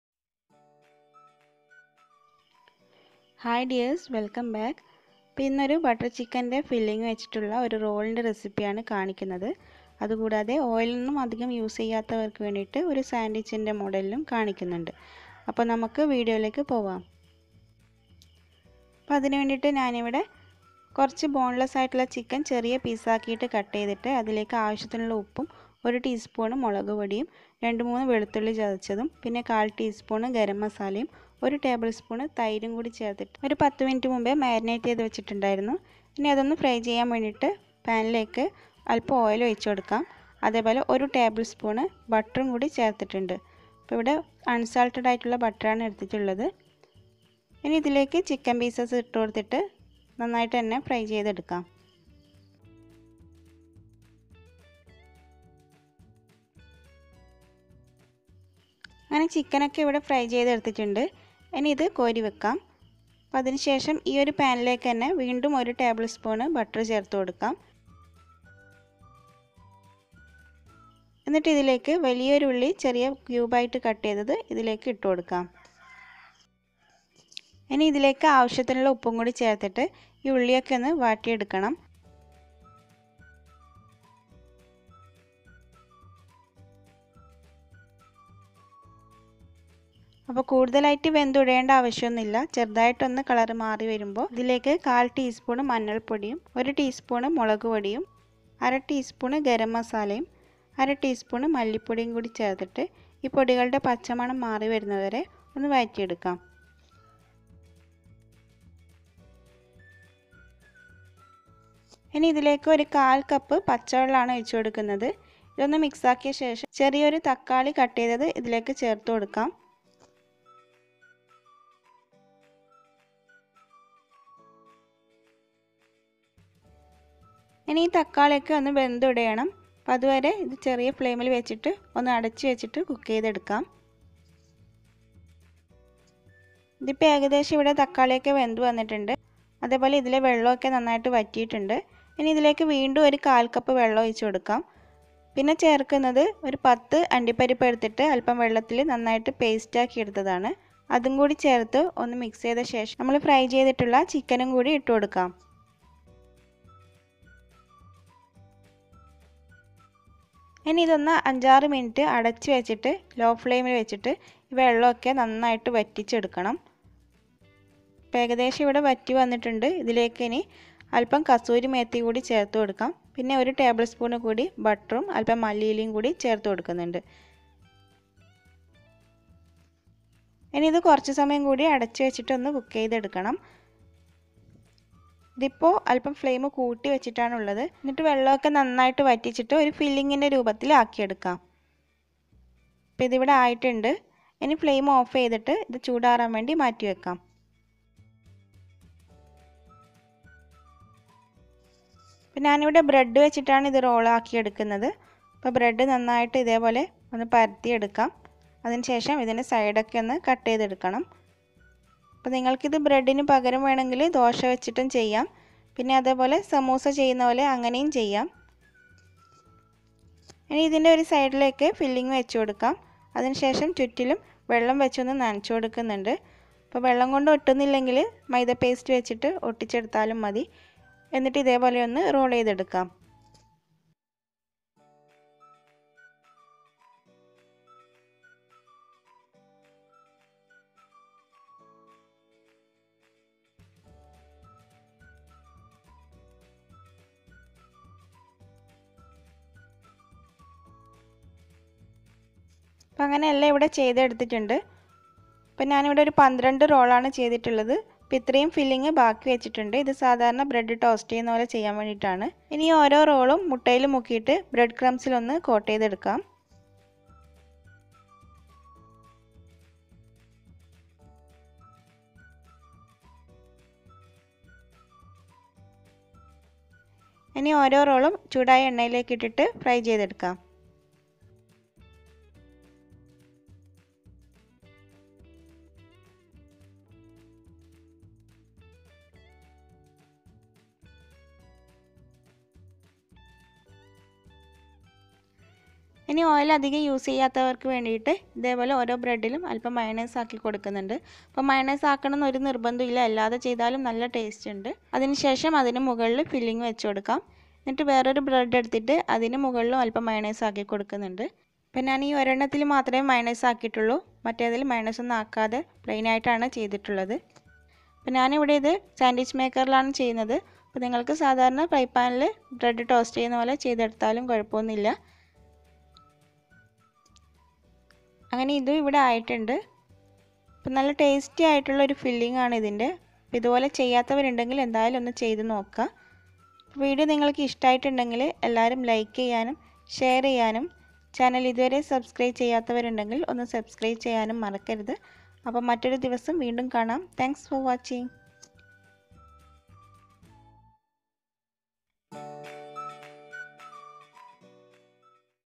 ഇനി റോൾ ഉണ്ടാക്കുമ്പോൾ ഇങ്ങനെ ചെയ്തു നോക്കൂ |Butter chicken bread roll | sandwich | Ummees kitchen
Chicken 150gm
Salt
Turmeric 1/2tsp
Chillipowder 1tsp
Curd 1tbs
Garlic 2

Butter3tbs
Oil 1tbs
Onion 1big
Ginger garlic
Turmeric 1/4tsp
Chilli powder 1tsp
Corriander powder 1/2tsp
Garam masala 1/2tsp
Kasurimethi
Tomatto 1small
Cashew paste/fresh cream